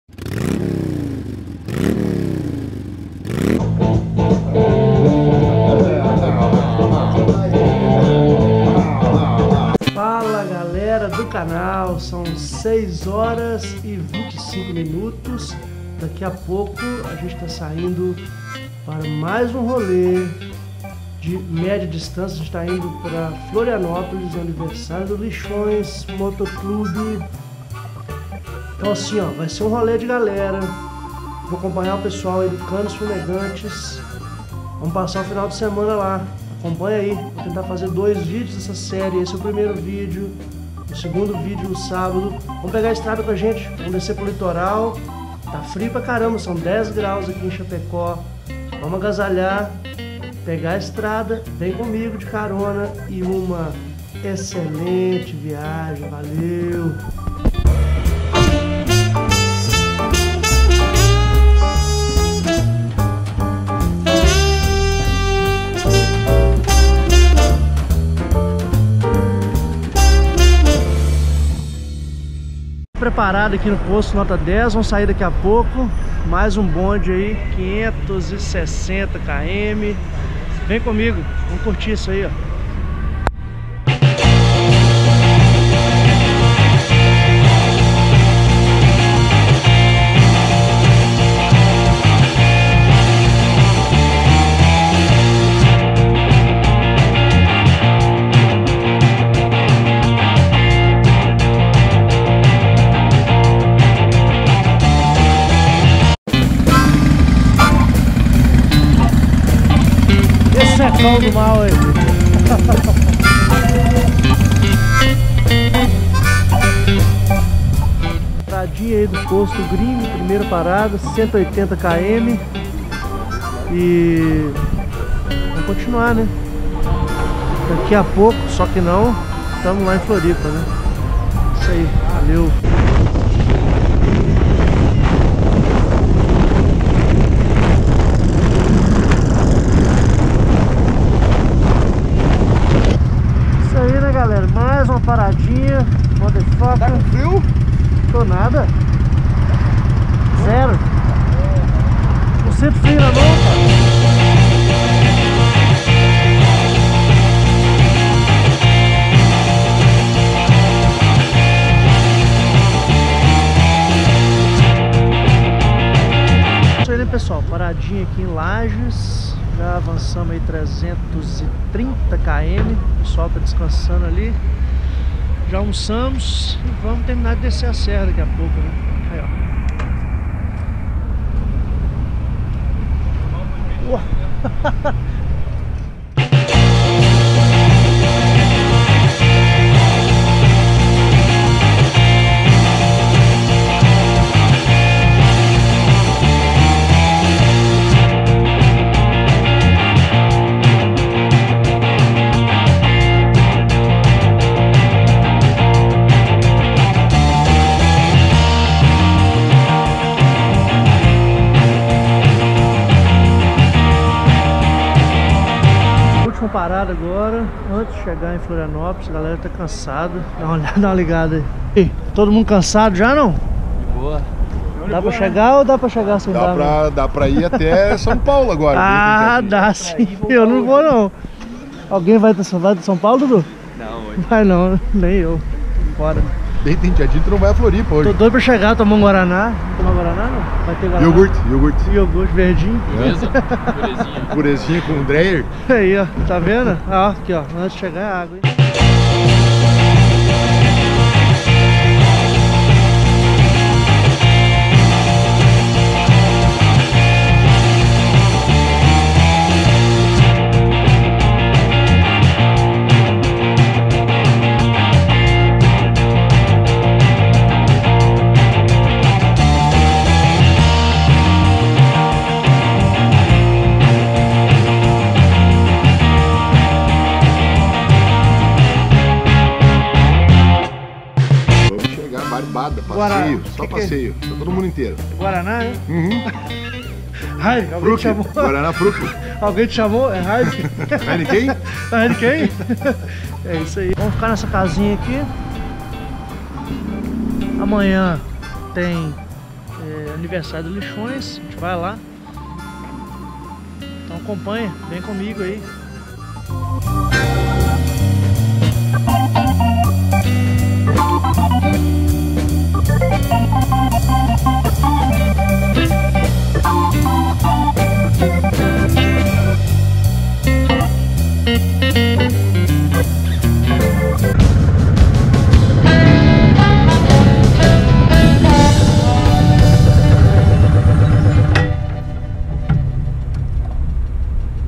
Floripa Feriado, Lixões - Parte I
Esse feriado viemos a Floripa no aniversário do Lixões MC, confira o que rolou em dois vídeos super legais.